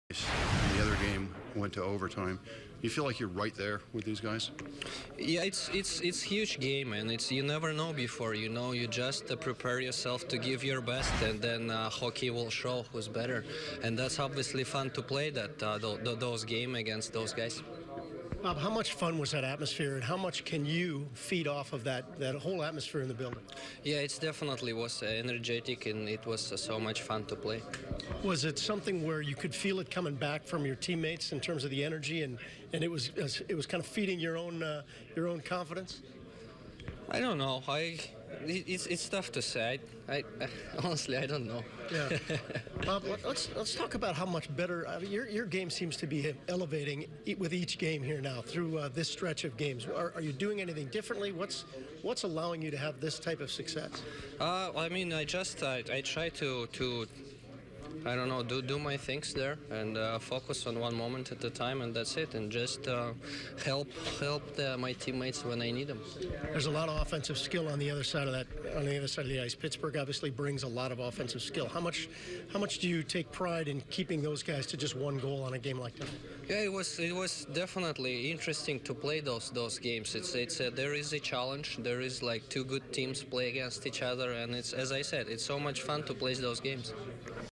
Sergei Bobrovsky helps lead the Columbus Blue Jackets to win over Pittsburgh Penguins
Bobrovsky saved 38 out of 39 shots to lead the Jackets to an overtime win over the Pens